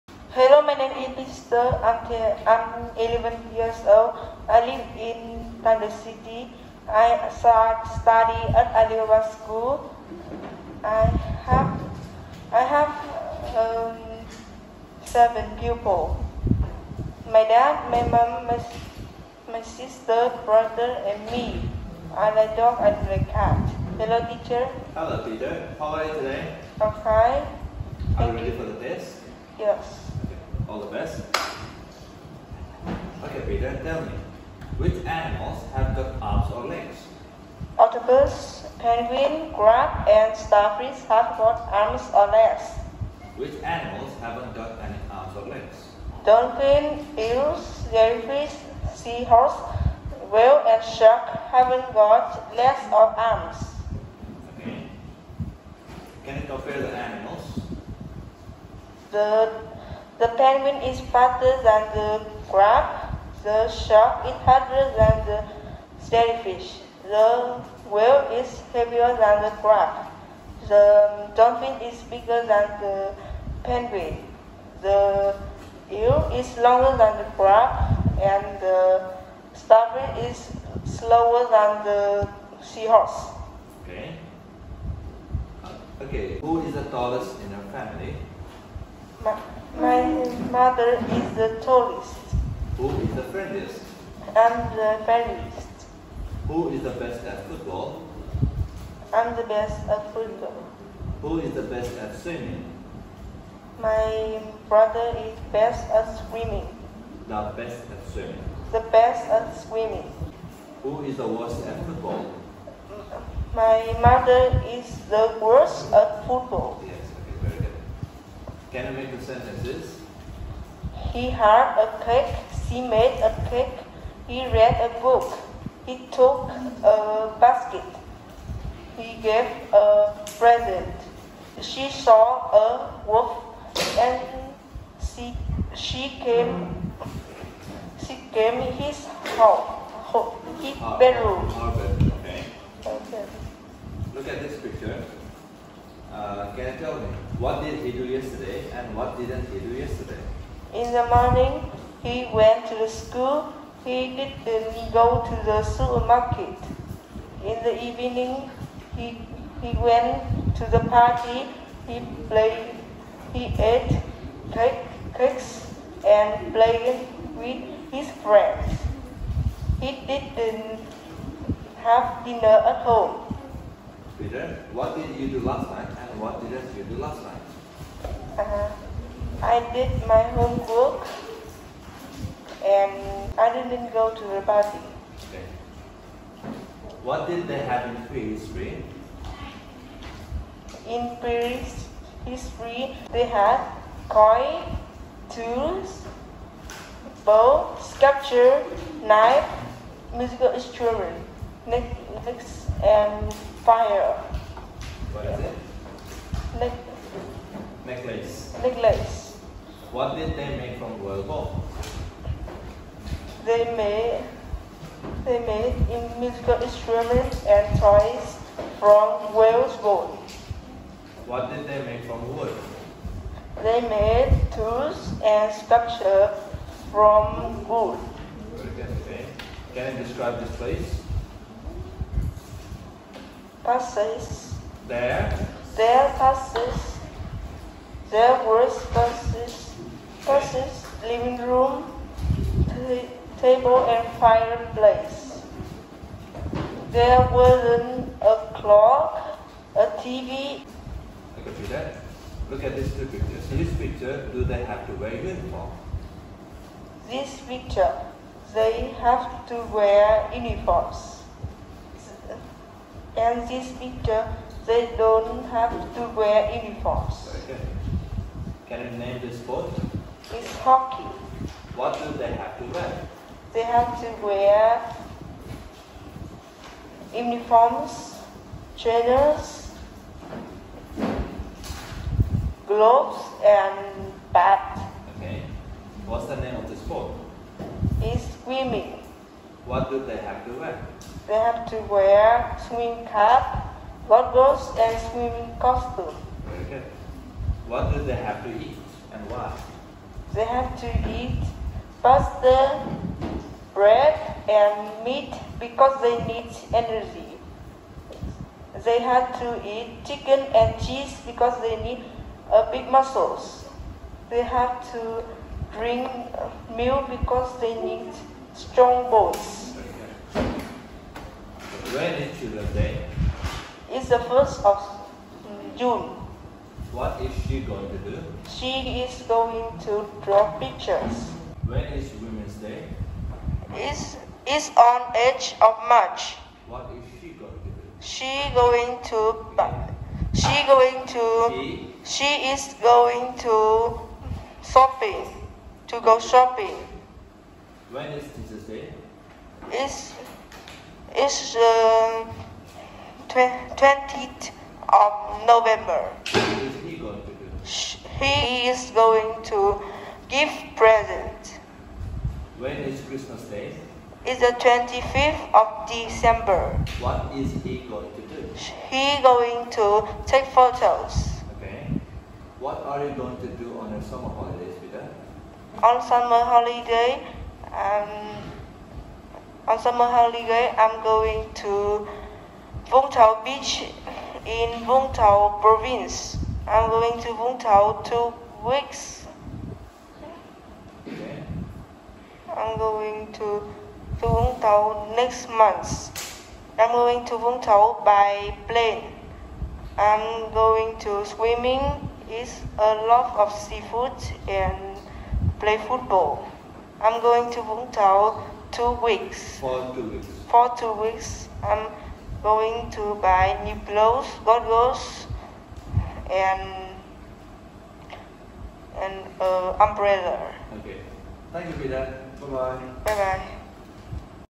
MẠNH VŨ - PETER - MOVERS B3 - ALIBABASCHOOL
Chúc mừng Peter đã hoàn thành xuất sắc bài thi số 2 của cấp độ Movers B.
Con ghi nhớ rất tốt các chủ đề: động vật dưới nước, đồ vật trong bảo tàng và vật dụng trong gia đình, trang phục và các dụng cụ thể thao, ngày tháng của các lễ hội.
Con vận dụng linh hoạt kiến thức ngữ pháp để trả lời bài bao gồm: so sánh hơn, so sánh nhất, thì quá khứ đơn, đưa ra lời giải thích với because , tương lai gần với tobe going to, động từ khiêm khuyết với have to.
Về phát âm: con phát âm các động từ chia thì quá khứ, âm đuôi của các từ rất tốt. Tuy nhiên, con lưu í từ: necklace, passage nhé!
Chúc mừng con!